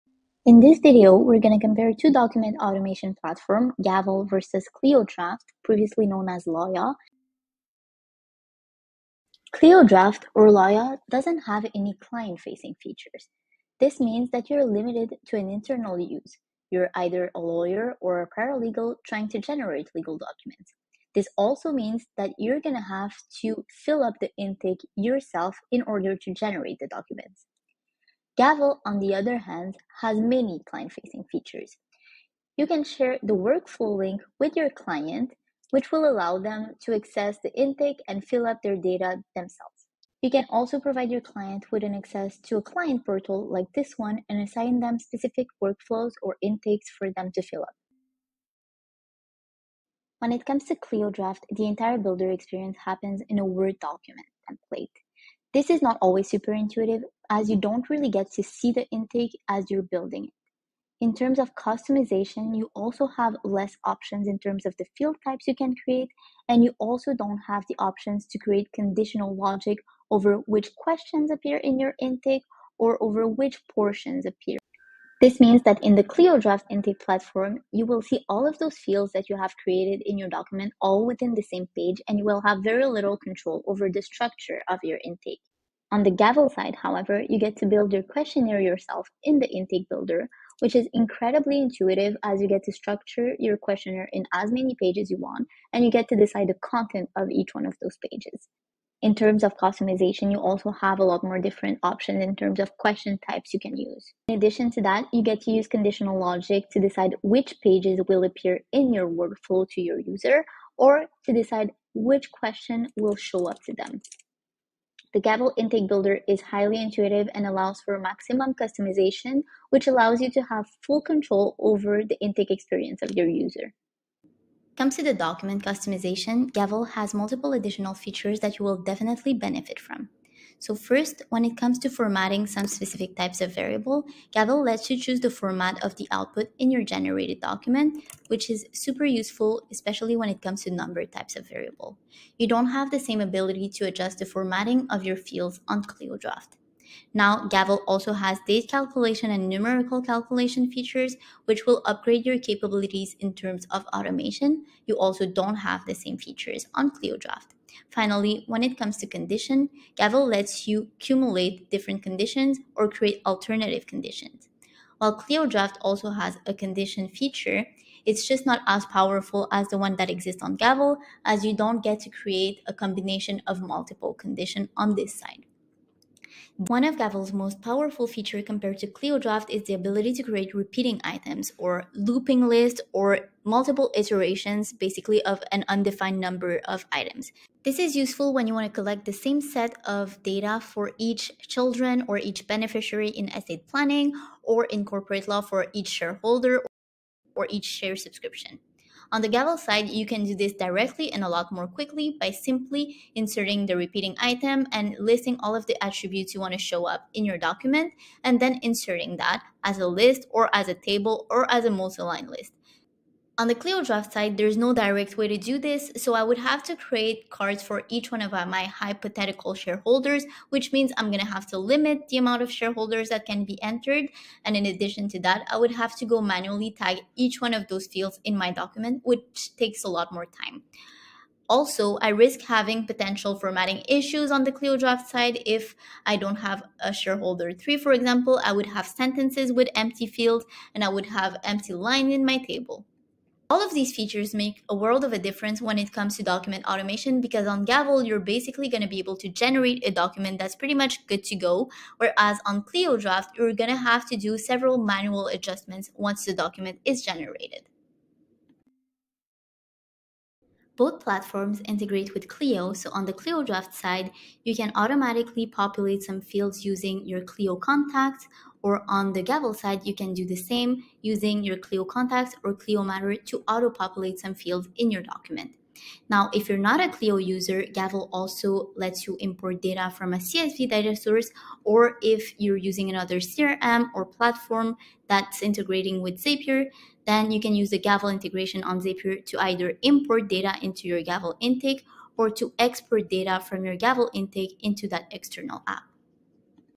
Clio Draft (previously LawYaw) v. Gavel: Feature Comparison
This video shows the differences between Clio Draft (formerly LawYaw) and Gavel, and why you would choose one platform over the other for your legal document automation needs.